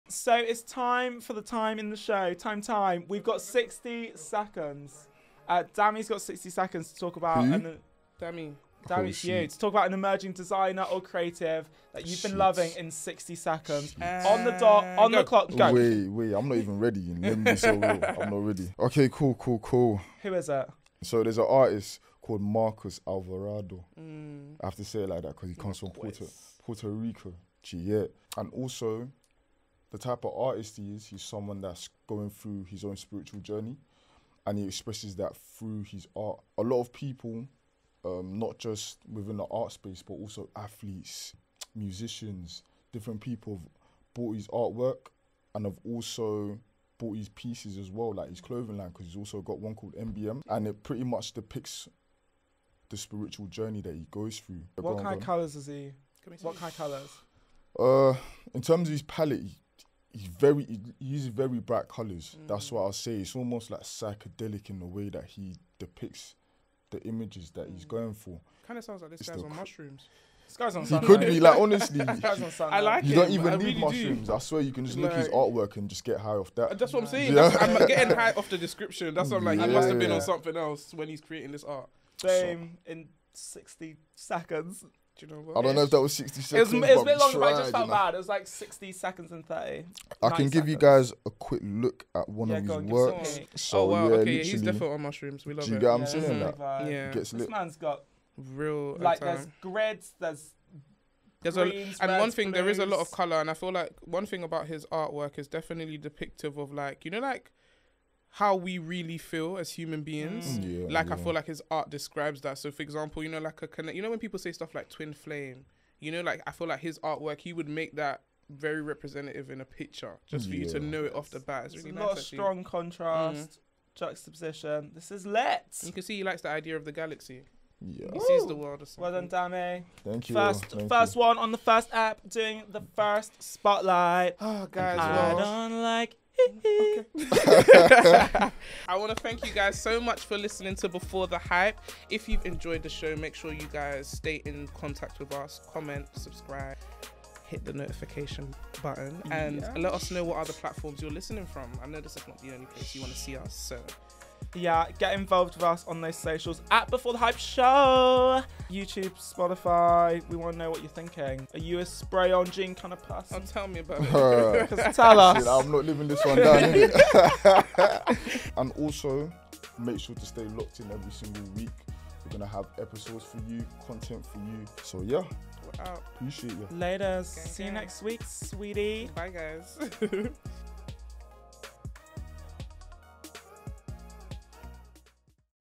He Must Be On Mushrooms! | Before The Hype Show
It's Dami's spotlight of the week and he is talking about designer and artist Marcus Alvarado and his trippy creations.